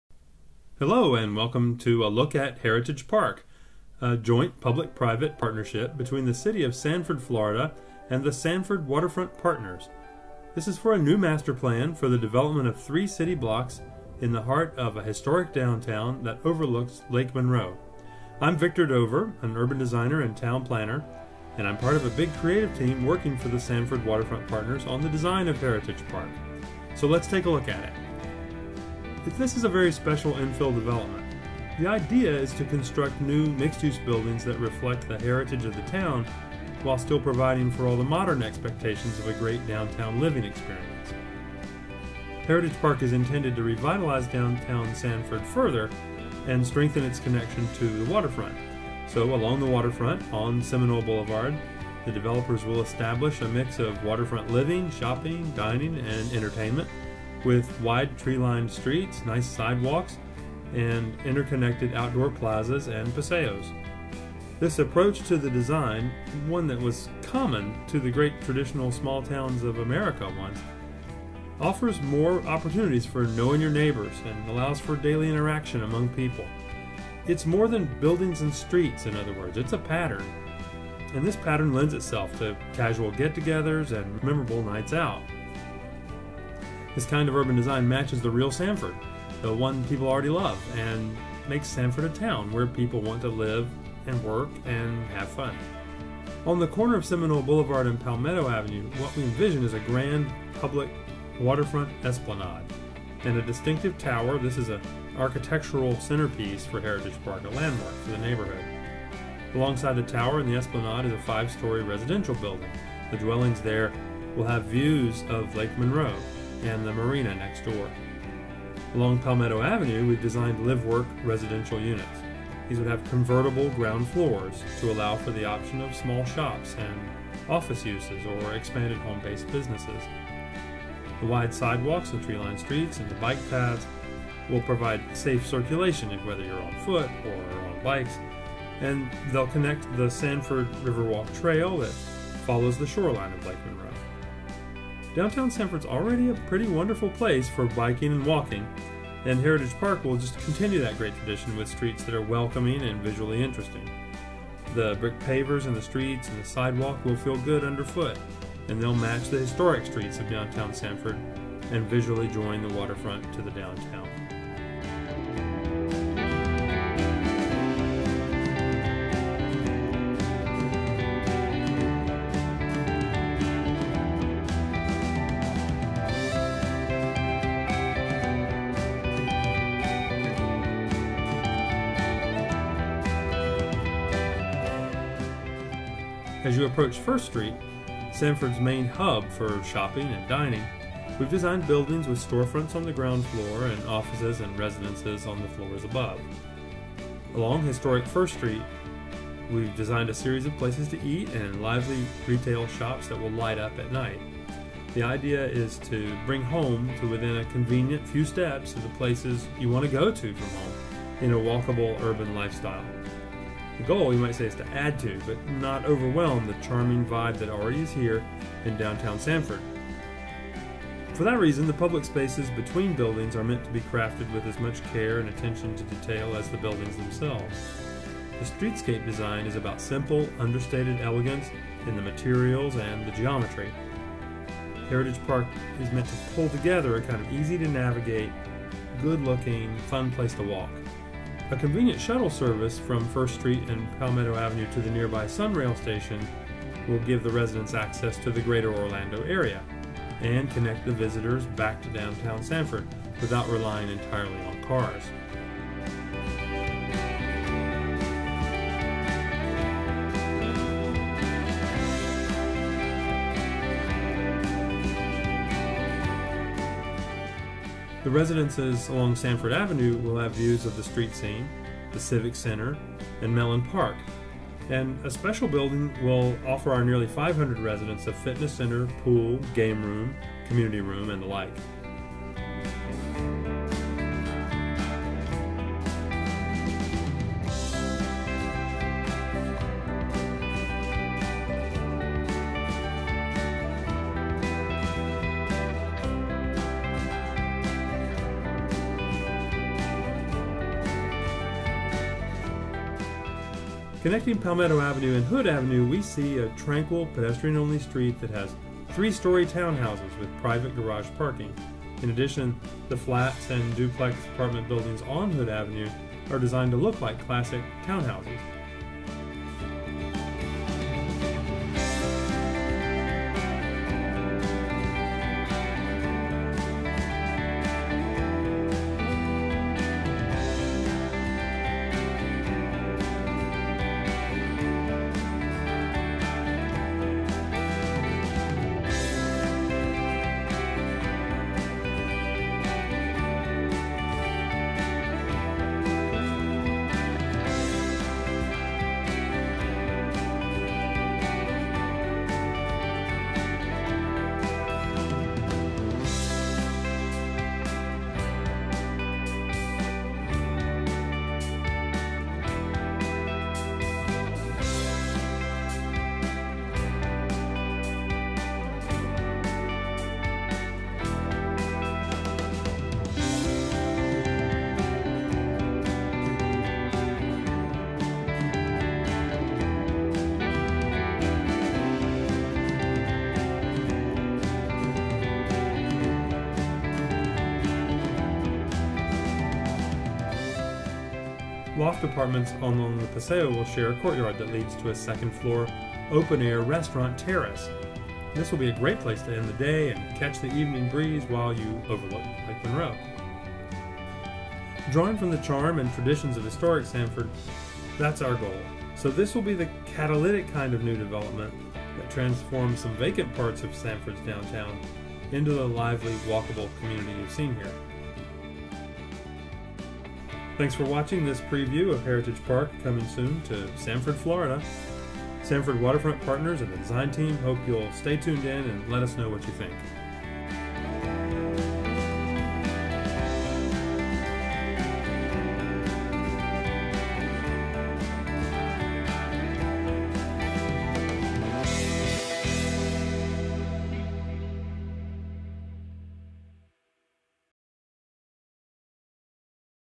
Heritage Park - Sanford, FL
Video walkthrough of preliminary plans for Heritage Park in Downtown Sanford, Florida. A joint public-private partnership between the City of Sanford, FL and the Sanford Waterfront Partners. Urban design by Dover, Kohl & Partners.